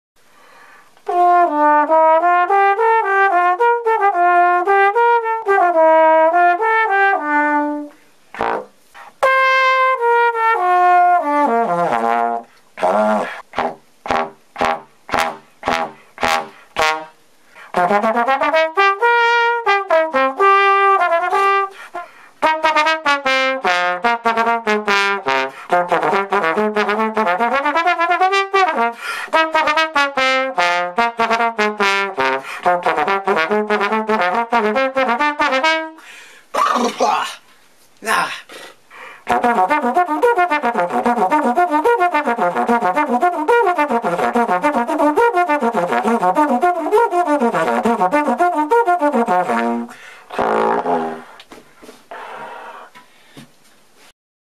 Trombone practice clips (camera audio test)
Testing the audio on my video camera, which turns out to be flawless. :D